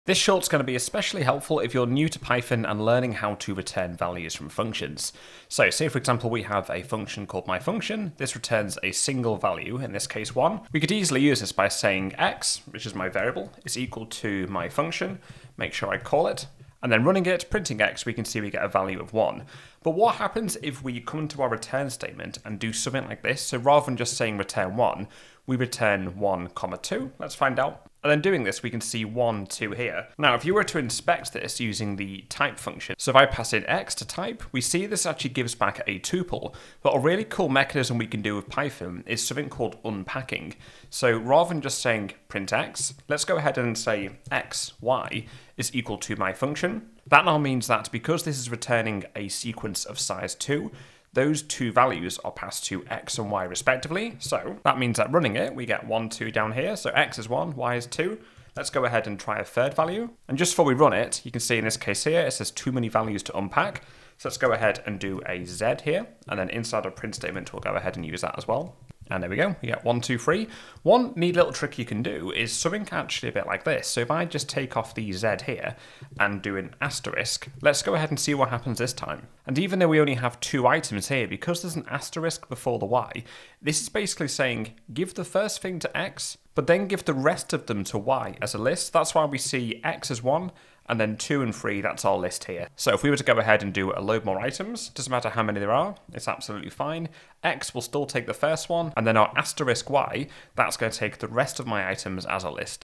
Python 3 | Funky ways of returning multiple items from your Function! coding
A beginners look at how to return multiple items from a function.